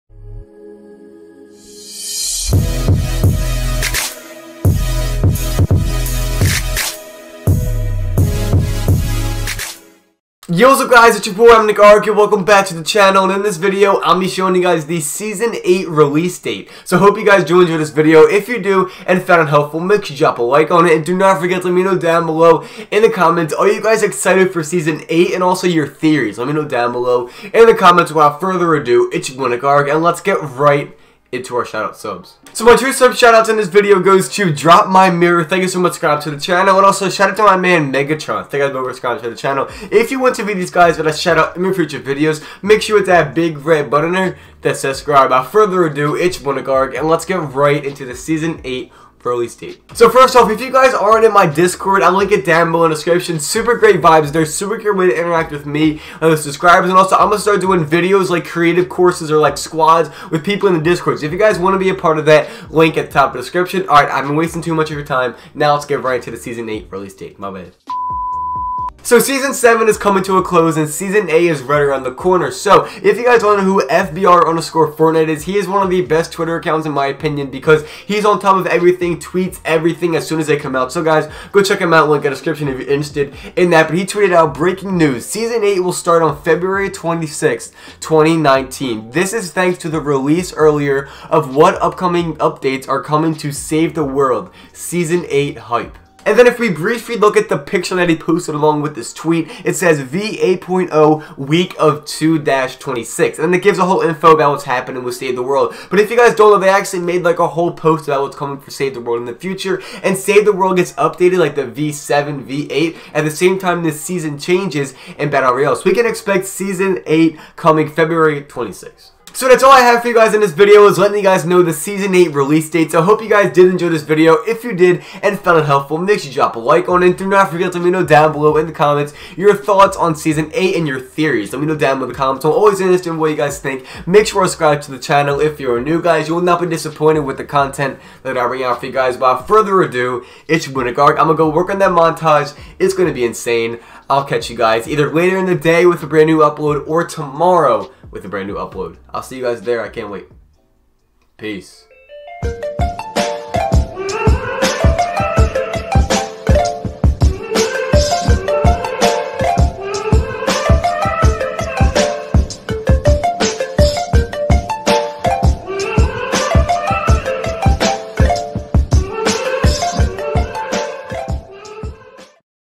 FORTNITE SEASON 8 RELEASE DATE START DATE! Fortnite Battle Royale 2019
The official release date of season 8 Fortnite battle royale it's gonna be awesome